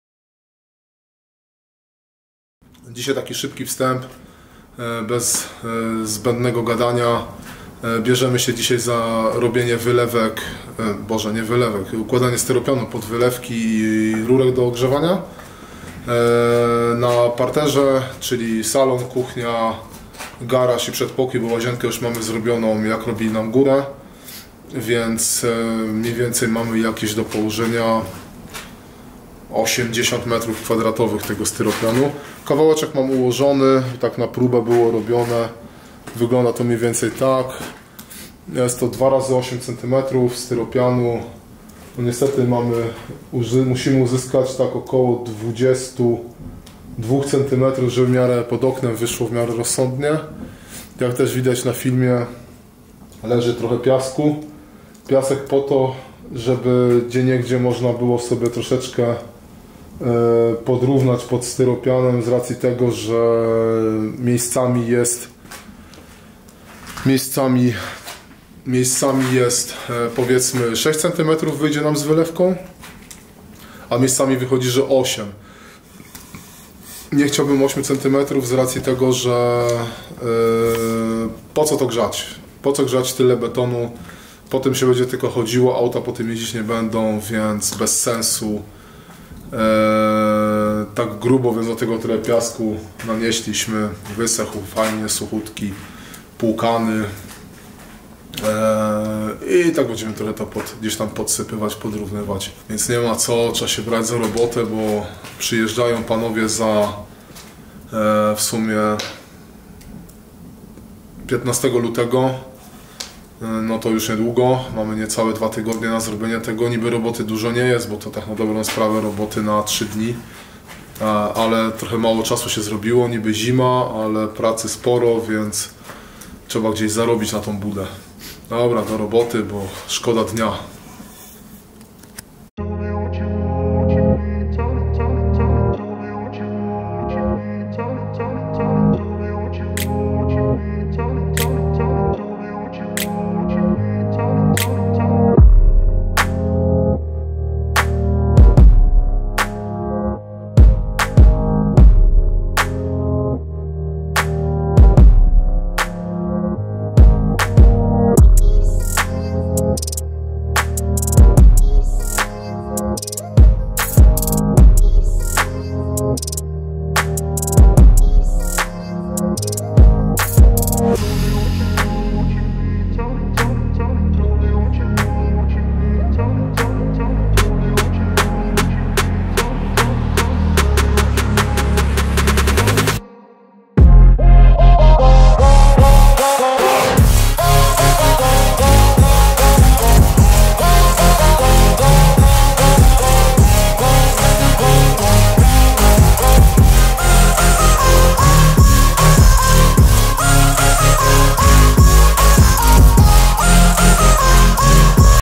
Ogrzewanie podłogowe, układanie rur PERT
W tym odcinku chciałem pokazać krótki filmik z układania rur pert i styropianu pod ogrzewanie podłogowe parteru. Niestety nie udało się wszystkiego nagrać ale w kolejnym odcinku opowiem co i jak i dla czego było tak zrobione.